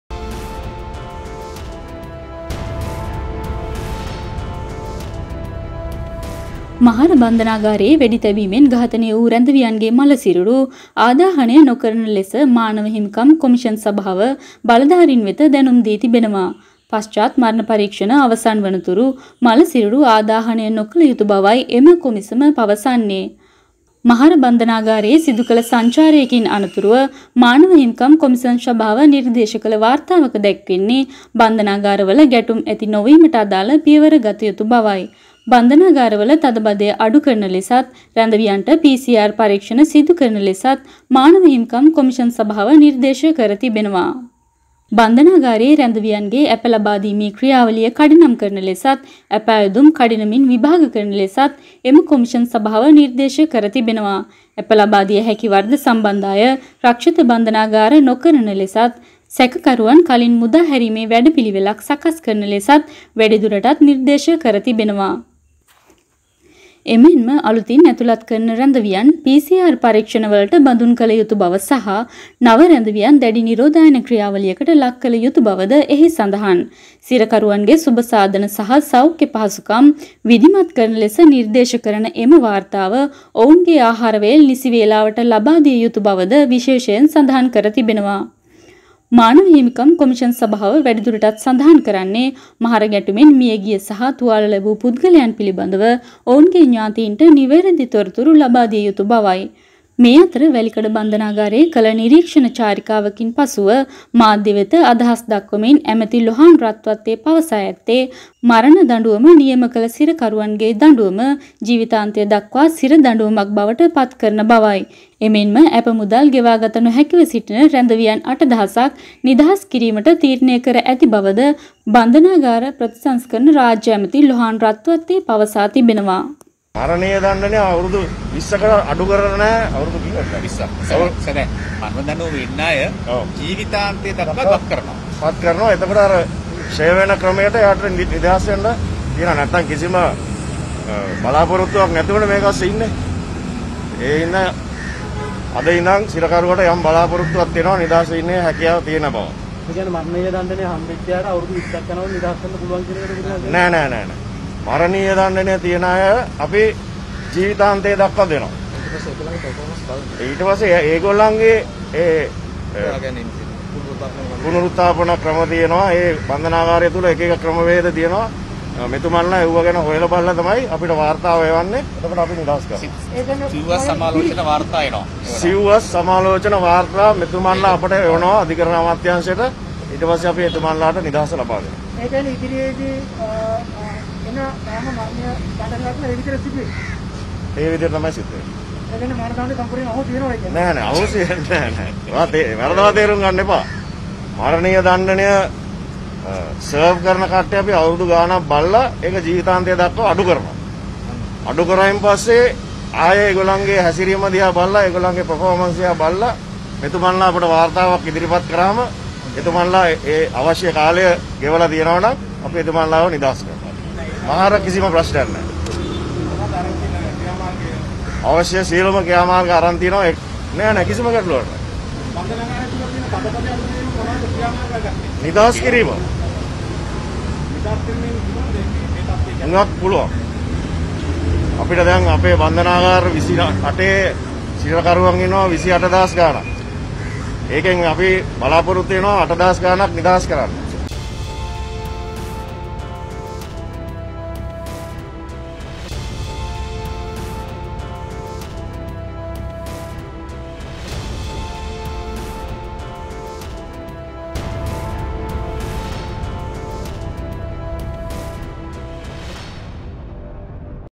Mass TV LK මහර බන්ධනාගාර ගැටුමෙන් පසු සිරකරුවන් 8000කට නිදහස - මළ සිරුරු ආදාහනයටත් තහංචියක්
මහර ගැටුමේදී ඝාතනය වූ සිරකරුවන්ගේ ‌දේහ ආදාහනය ‌නොකරන ‌ලෙස මානව හිමිකම් ‌කොමිසම දැනුම් දී තිබෙන අතර මරණ දඬුවම නියම කර තිබෙන සිරකරුවන්ගේ දඬුවම ජීවිතාන්තය දක්වා ‌ලෙස ‌වෙනස් කිරීමට තීරණය කර තිබෙනවා. ‌එමෙන්ම සිරකරුවන් 8000ක් නිදහස් කිරීමටද තීරණය කර ඇත.